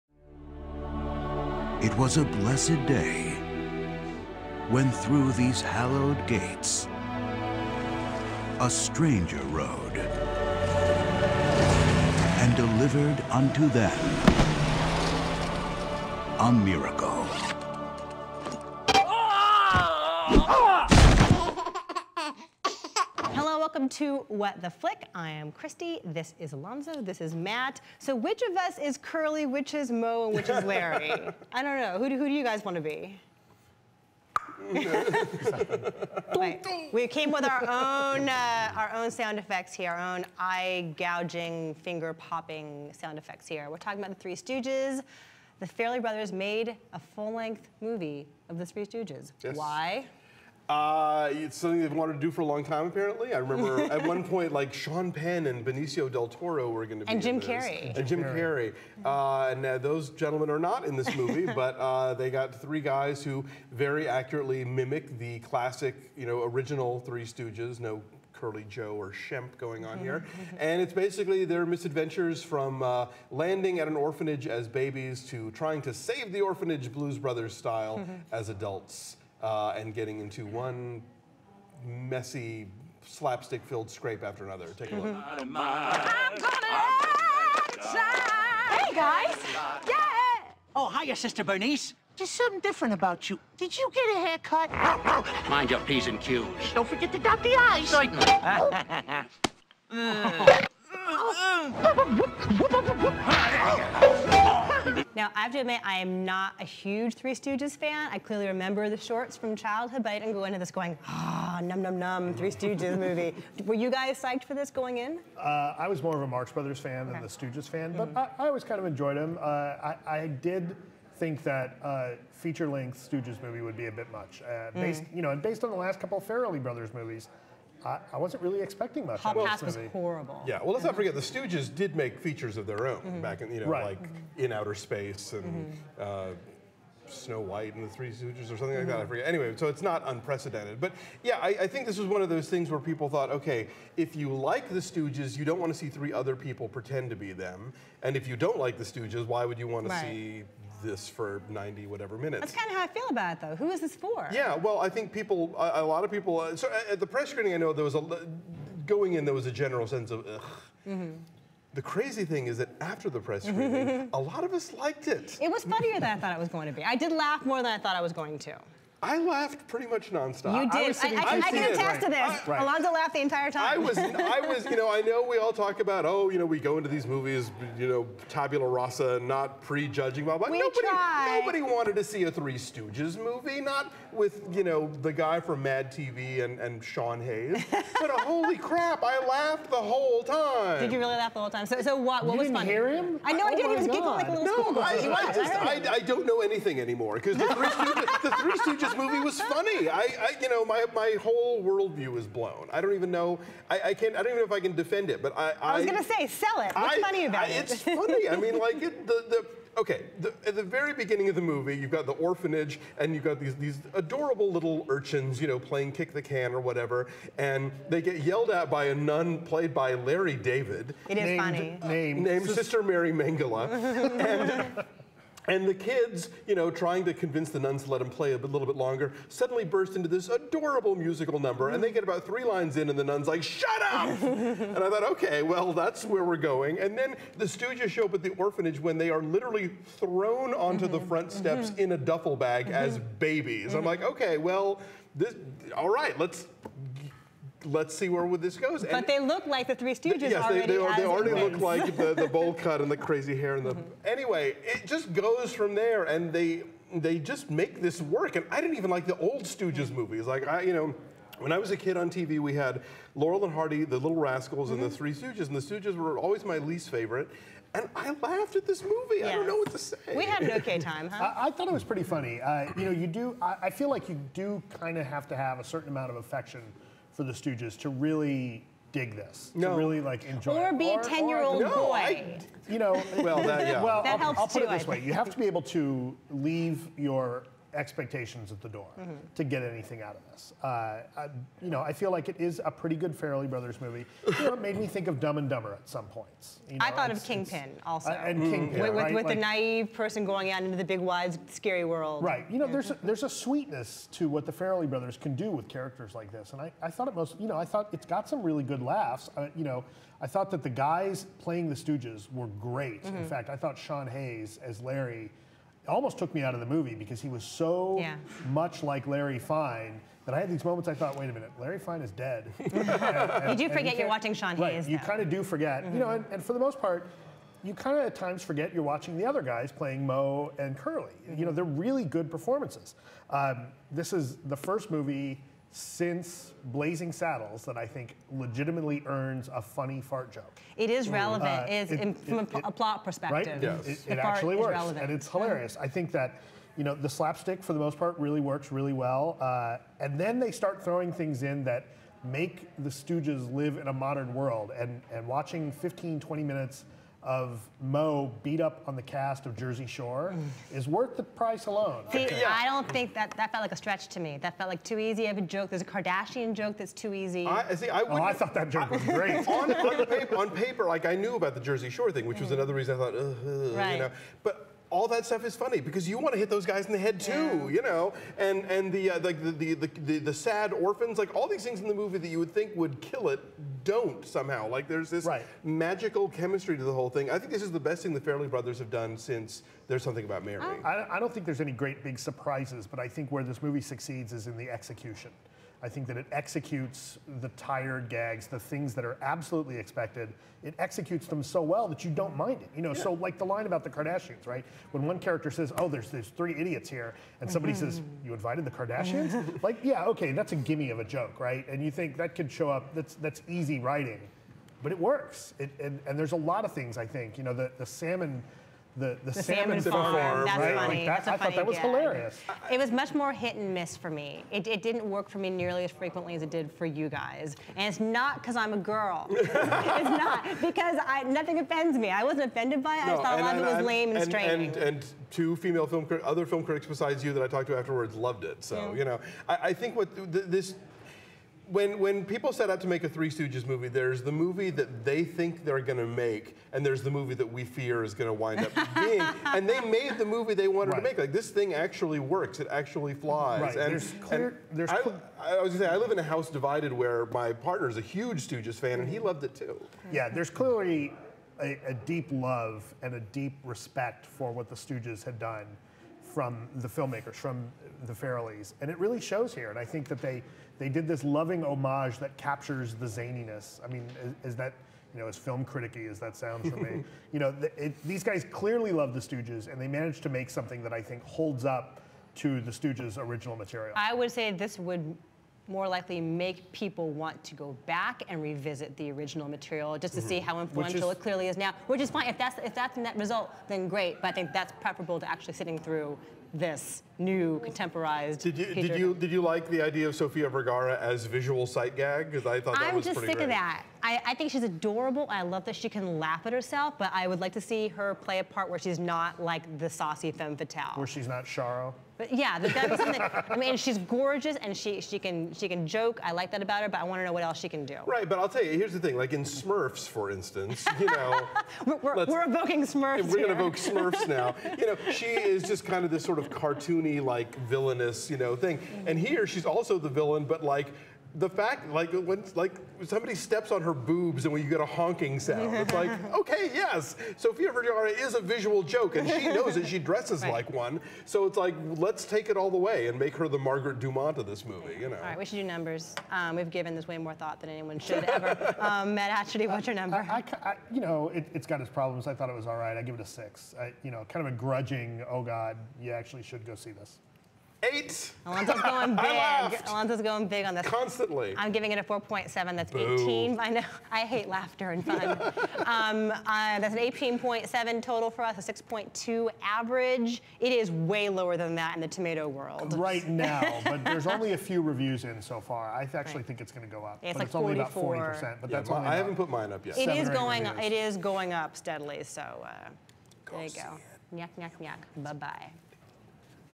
The Three Stooges - Movie Review
The Three Stooges written and directed by the Farrelly Brothers is reviewed by Christy Lemire (AP critic) Alonso Duralde (TheWrap.com and Linoleum Knife podcast) and Matt Atchity (Editor-in-chief Rottentomatoes.com).